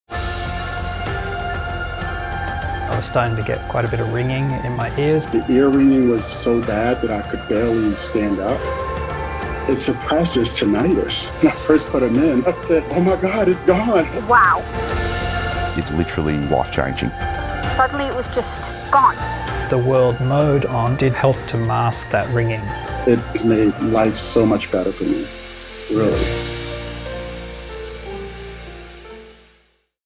Tinnitus Relief with IQbuds² MAX
Tinnitus impacts approximately 15% of all people. Discover how Nuheara's IQbuds² MAX earbuds help alleviate the persistent ringing, buzzing, and other symptoms of tinnitus.